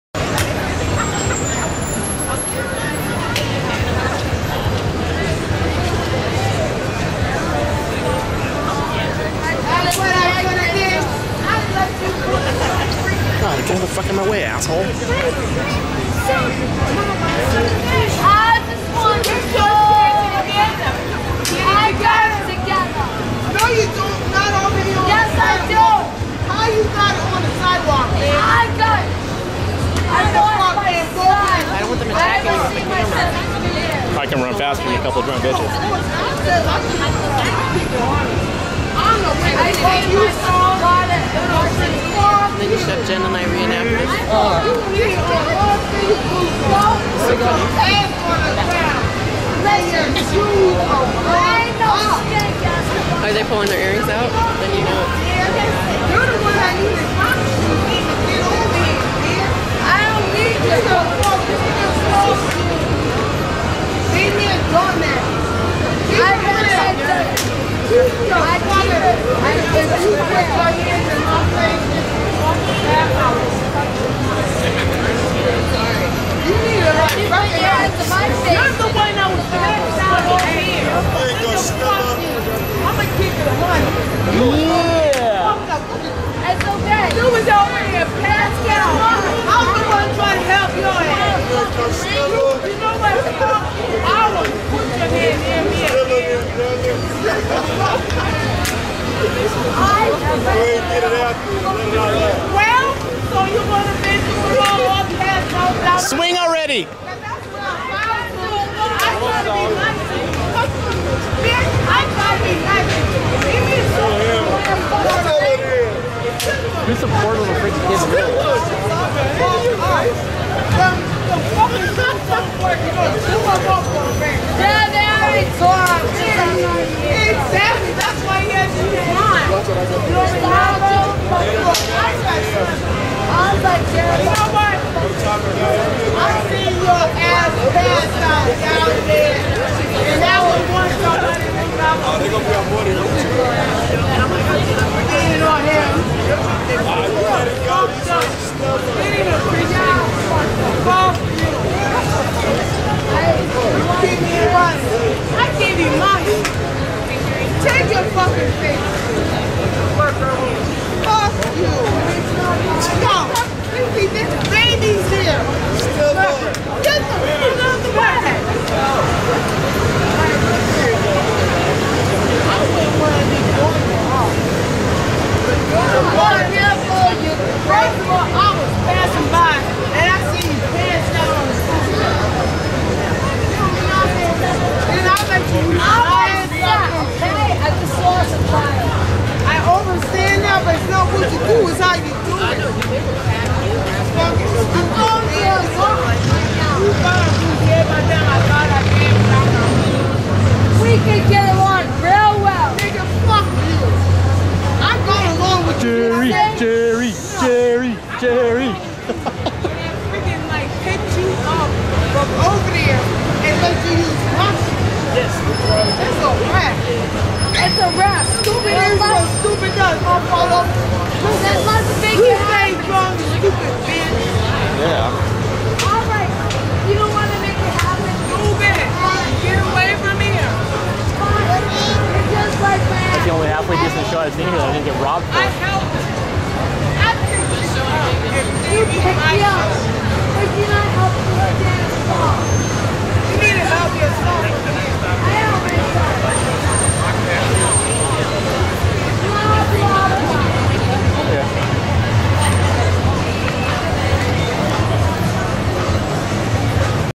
Hooker Turf Battle on Las Vegas Strip with Teenage Pimp Caught in the Middle
Two hookers get into a heated turf battle in front of Paris Casino on the Las Vegas Strip. Teenage pimp gets caught in the middle.